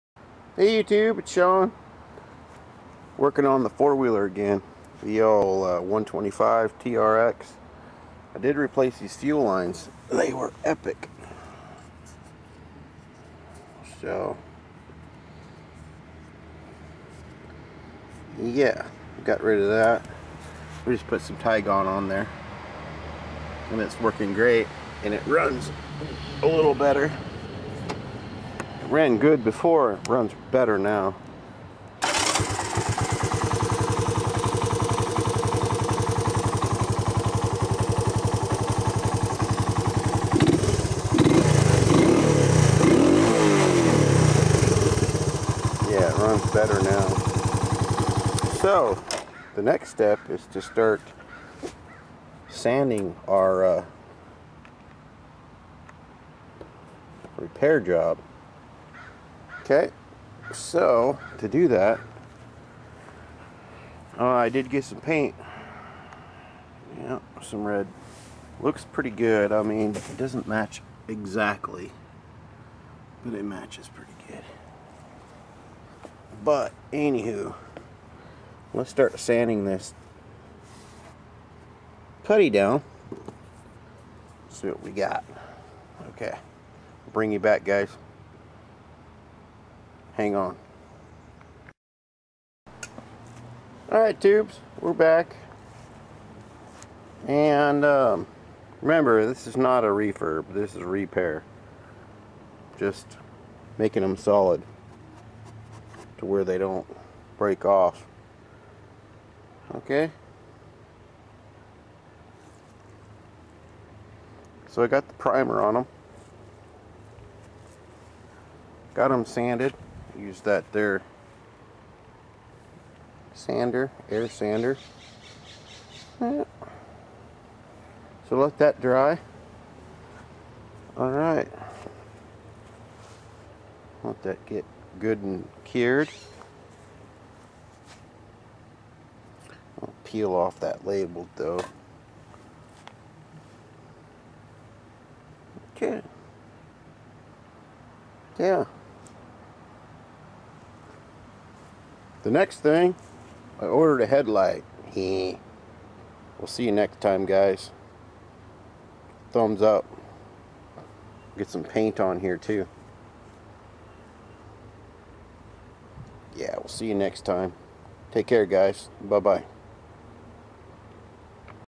Honda TRX 125 Repair (PART 4 )
A little fender work , Much more to do !